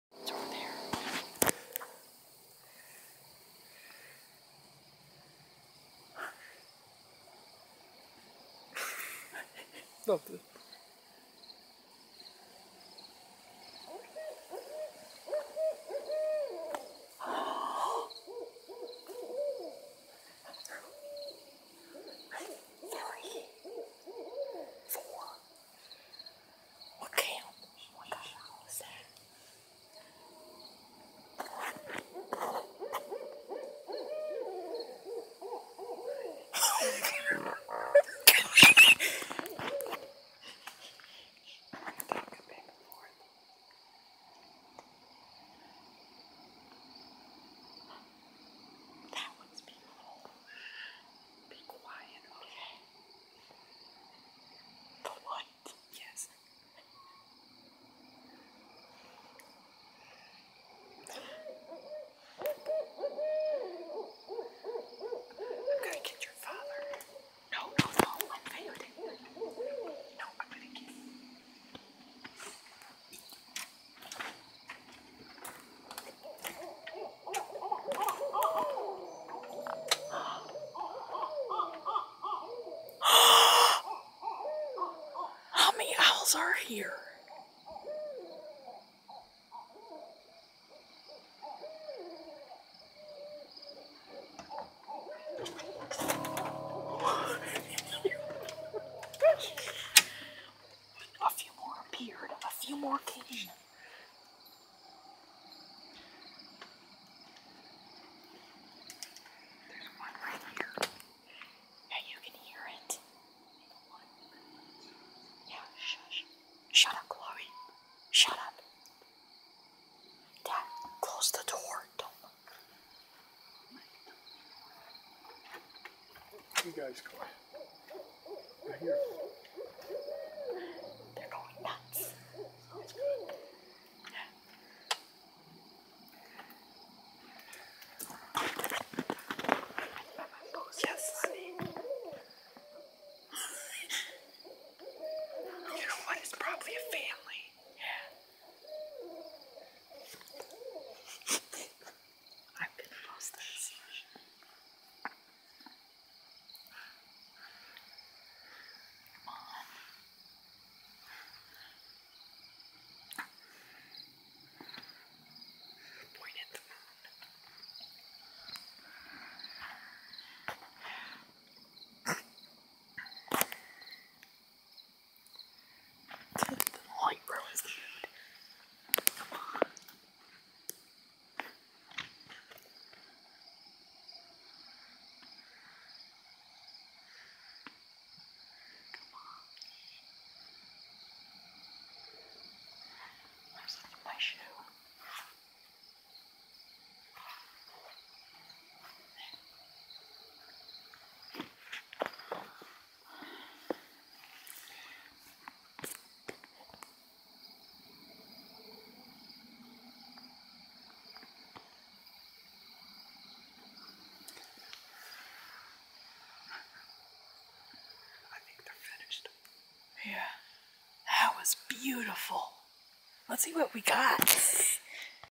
Owl sighting with my mom! Listen...
Zero two, me and my visiting mom, heard some owls going nuts!